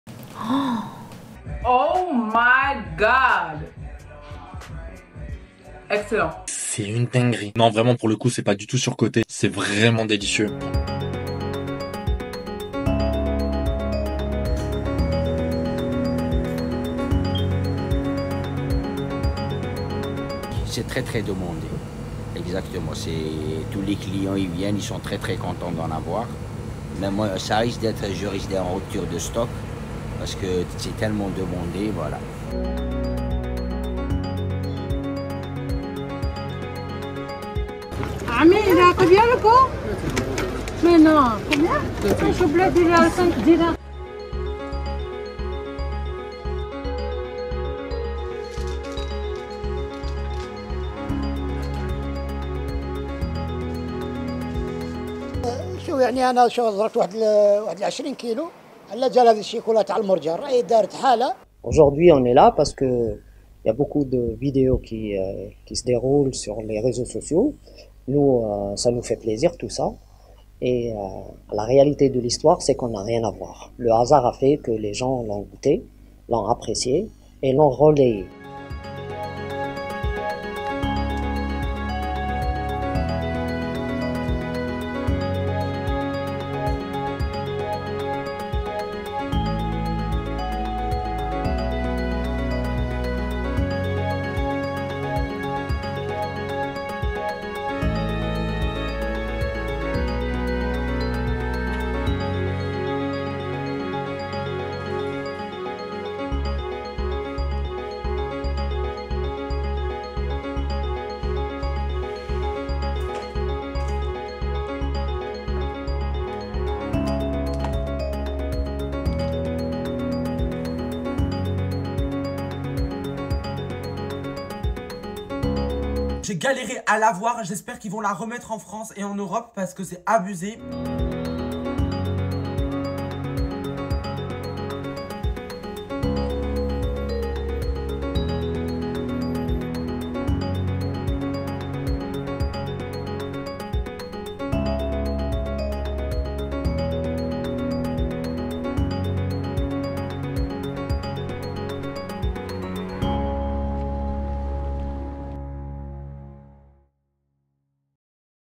El Mordjene, la pâte à tartiner algérienne bannie des rayons français • FRANCE 24
Le buzz autour de la pâte à tartiner algérienne El Mordjene Cebon fait grimper son prix en France.